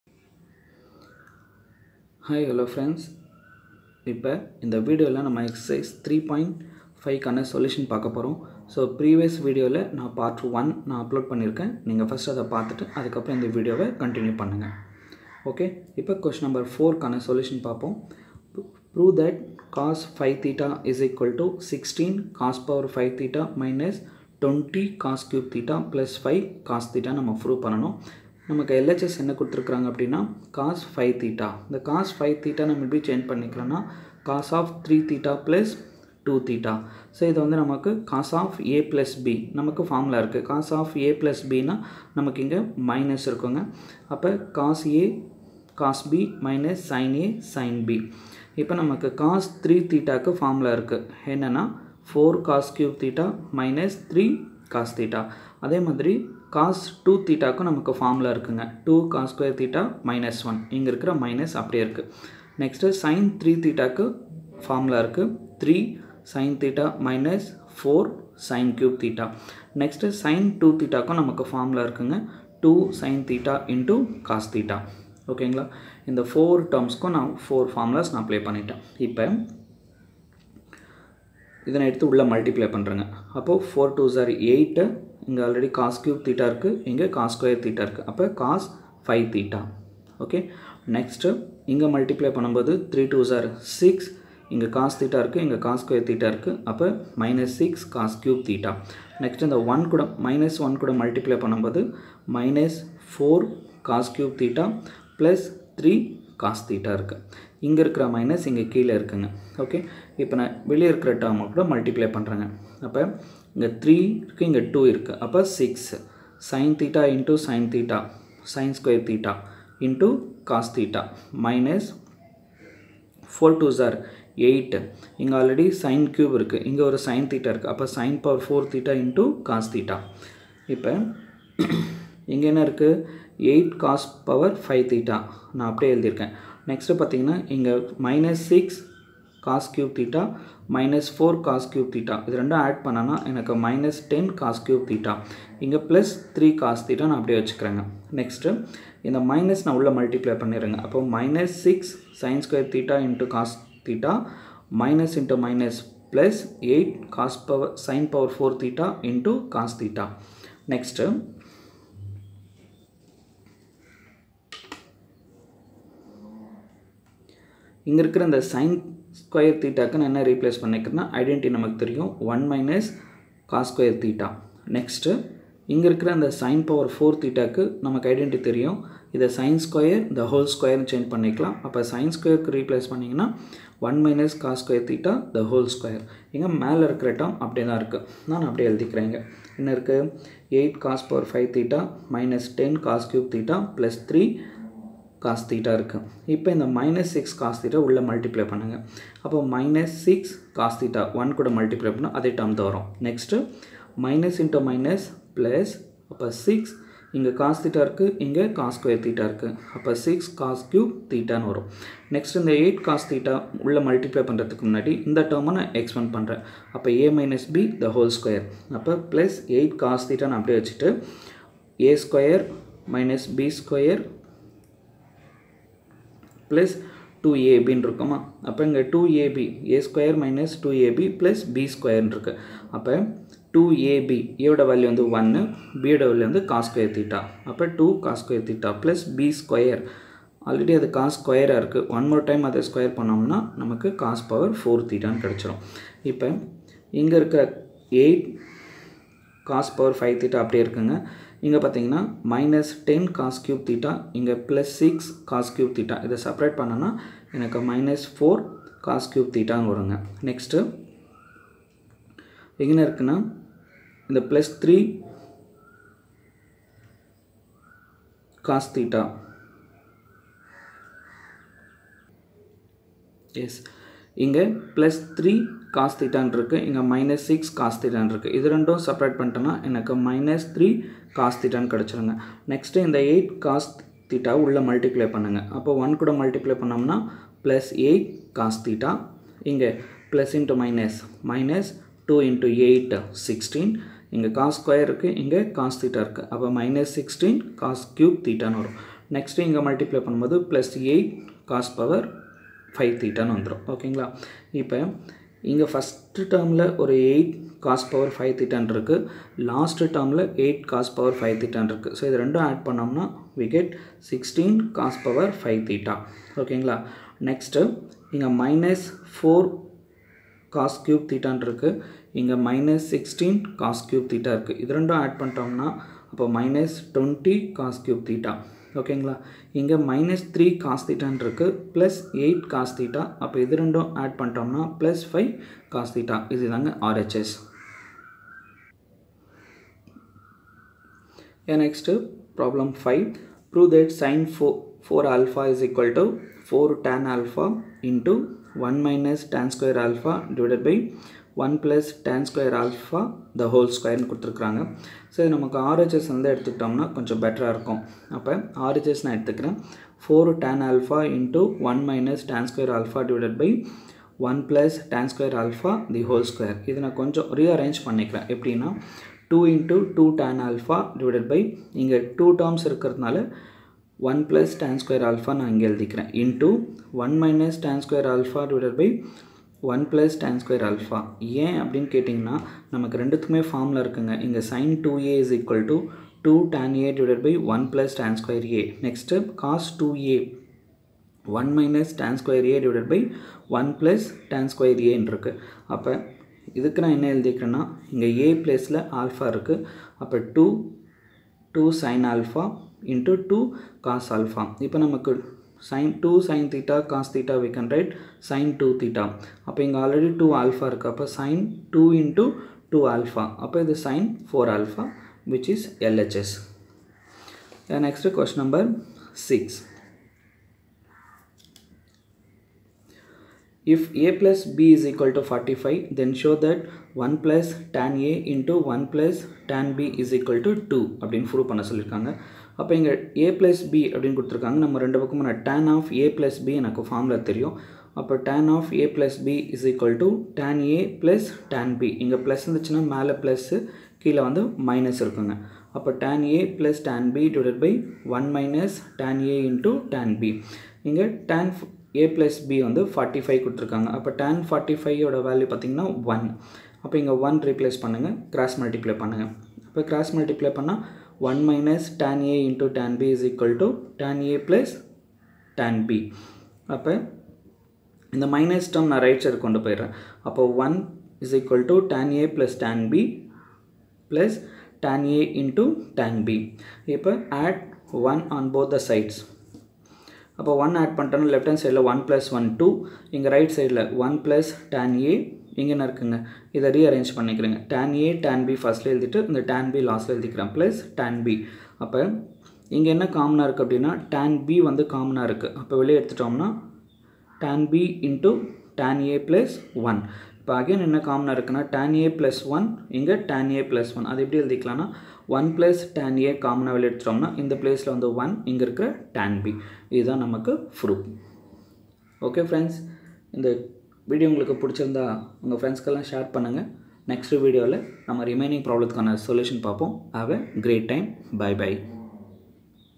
Class 11 Maths Unit 3 : Solved Problems in Exercise 3.5 Part 2
This is Part 2 of Solved Problems in Exercise 3.5

In this video explained Trigonometry Double angle & Half- angle identity problems in Exercise 3.5.